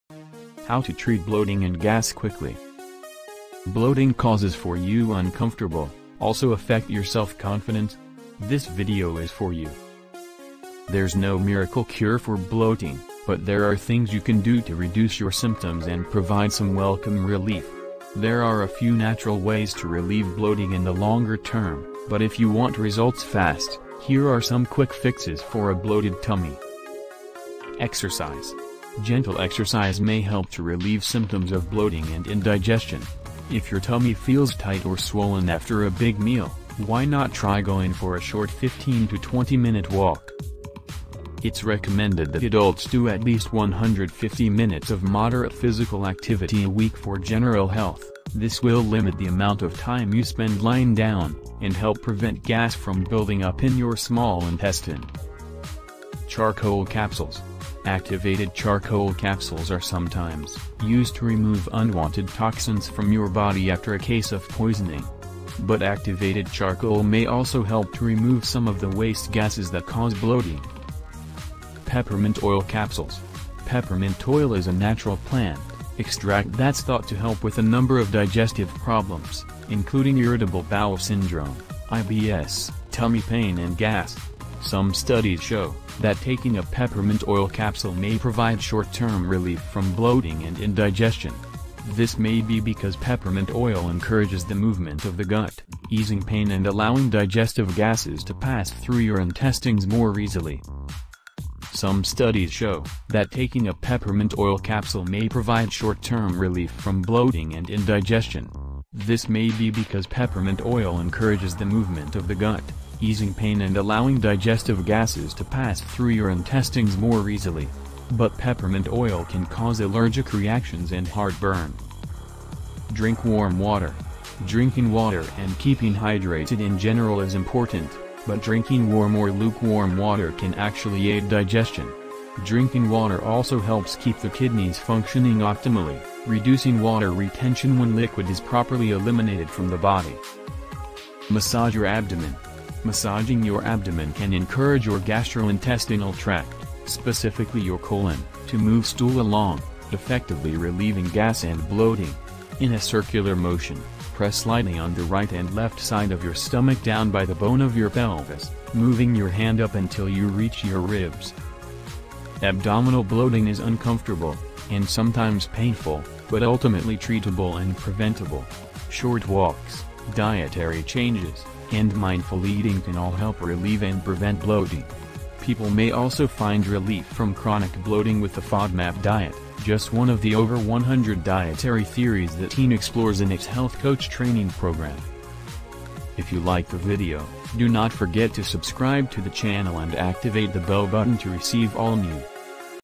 how to treat bloating and gas quickly/How to Stop bloating Fast,Natural Home Remedy .
how to treat bloating and gas quickly/How to Stop BLOATING .
Bloating can be uncomfortable, and it can also affect your self-confidence.

There’s no ‘miracle cure’ for bloating, but there are things you can do to reduce your symptoms and provide some welcome relief.

There are a few natural ways to relieve bloating in the longer term, but if you want results fast, here are some quick fixes for a bloated tummy, just show this video.

Abdominal bloating is uncomfortable – and sometimes painful – but ultimately treatable and preventable. Short walks, dietary changes, and mindful eating can all help relieve and prevent bloating. People may also find relief from chronic bloating with the FODMAP diet, just one of the over 100 dietary theories that IIN explores in its Health Coach Training Program.

Best ways to use healthy and glowing skin, If You Do this, Your Face Will Look Younger